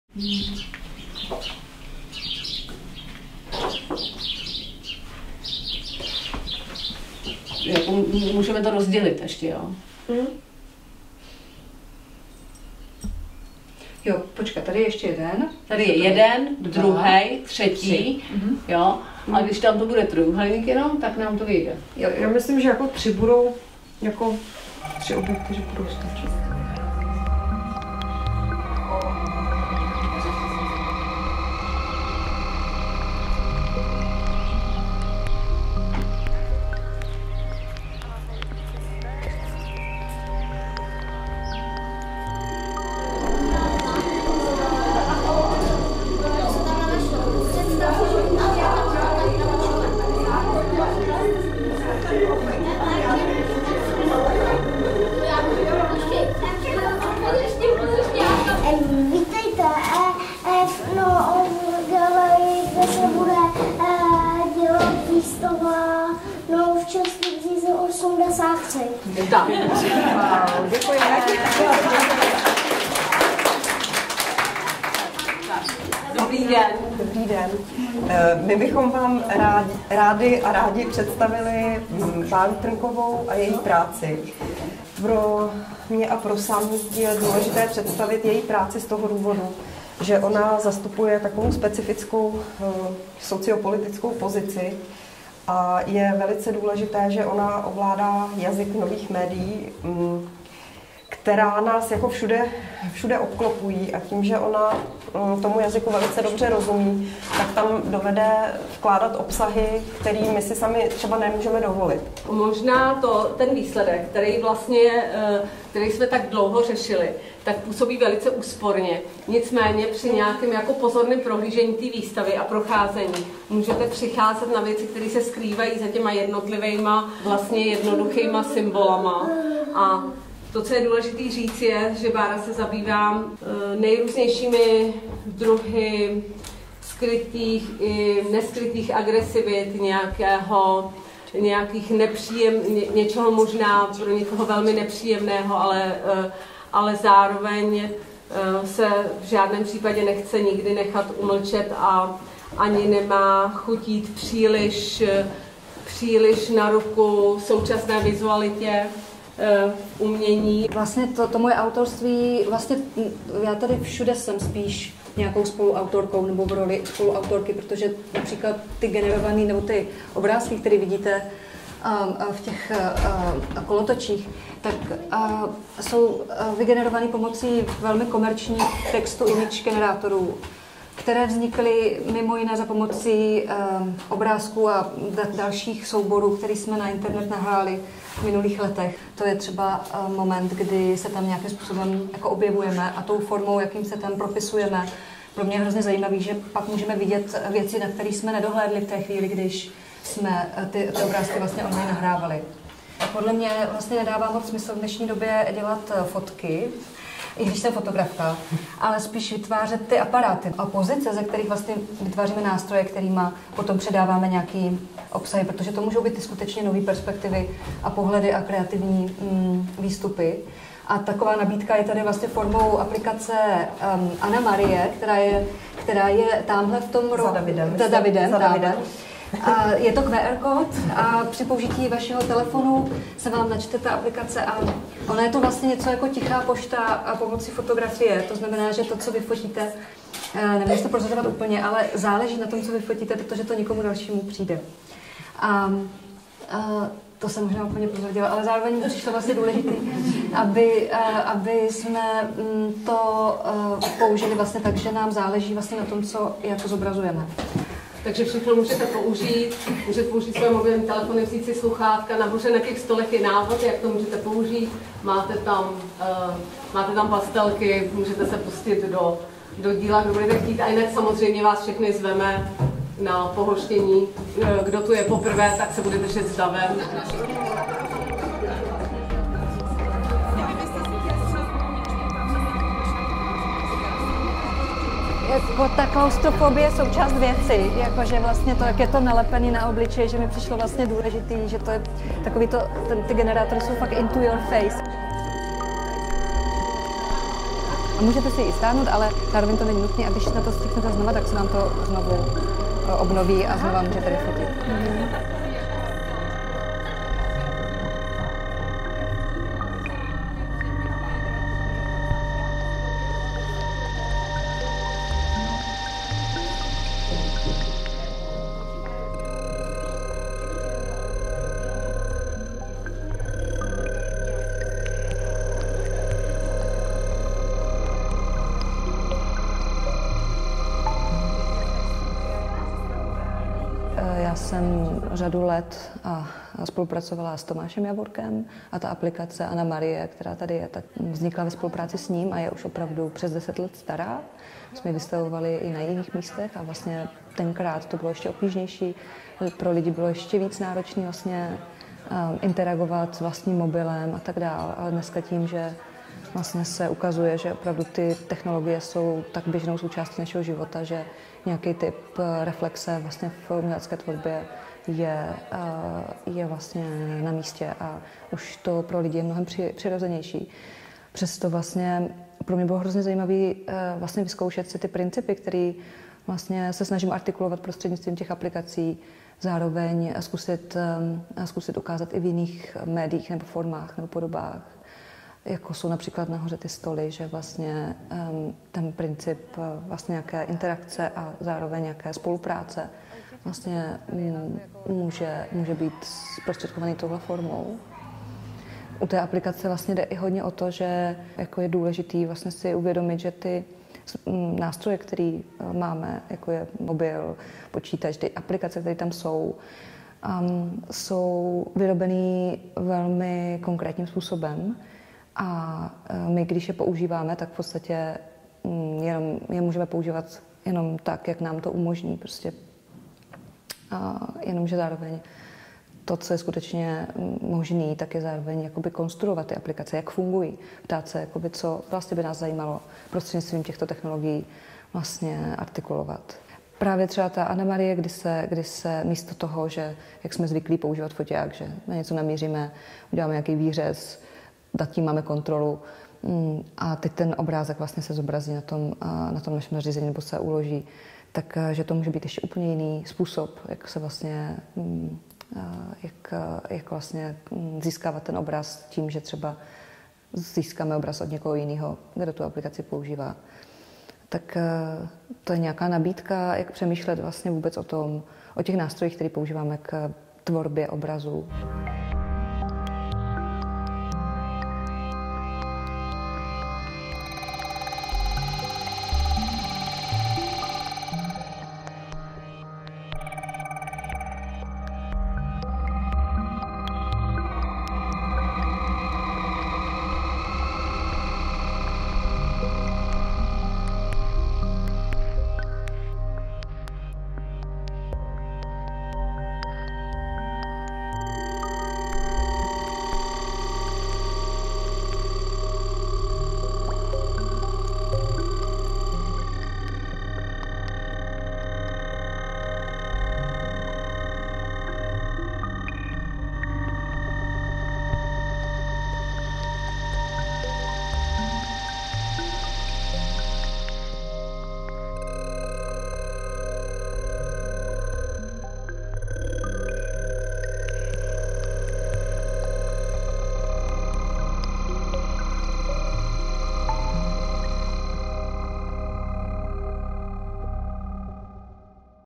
Barbora Trnková - Tváře vystupují z virtuální oblohy
Barbora Trnková je umělkyně, fotografka a výzkumnice v oblasti nových médií.
vernisáž 23. 3. / výstava 24. 3. – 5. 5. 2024
kurátorka: Sráč Sam
úvodní slovo: Denisa Bytelová